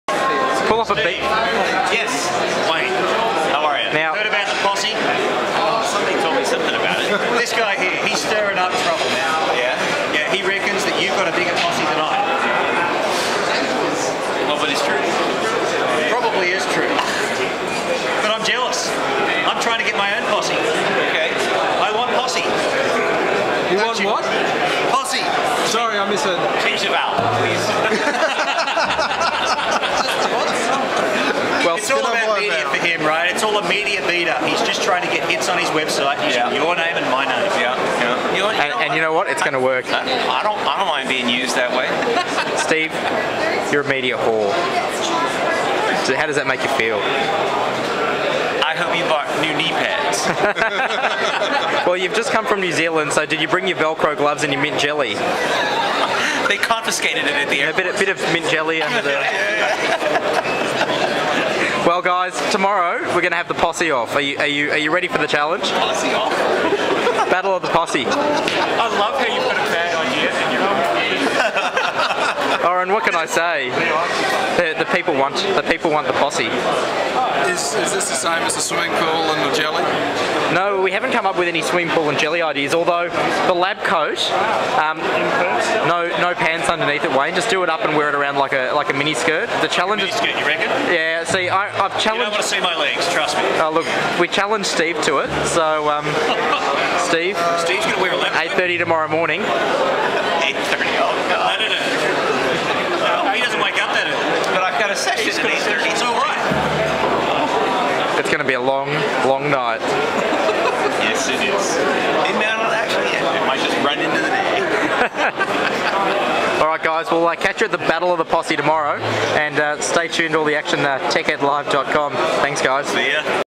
Small VS Riley... Let the games begin!
Wayne Small introduces himself to Steve Riley at Microsoft Tech.Ed 2008